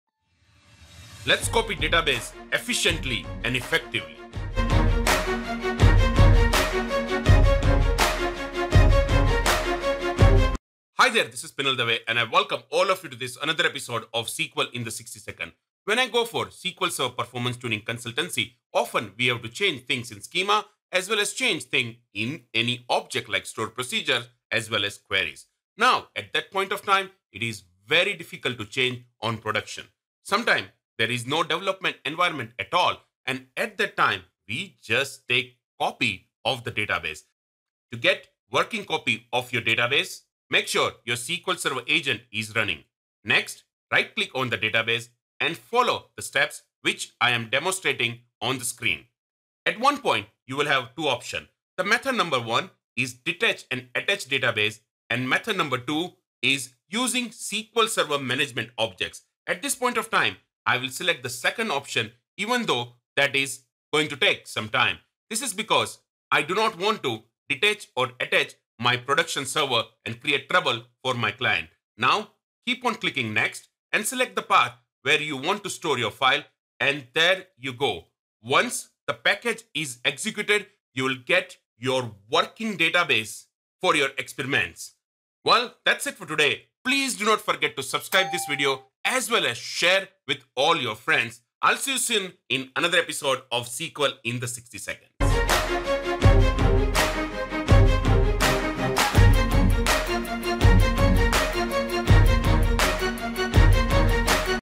Copy Database in SQL Server - SQL in Sixty Seconds 169
Let us learn today how to copy database in SQL Server in this quick video.

0:00 Intro
0:15 Real World Scenario
0:37 Demo
1:24 Summary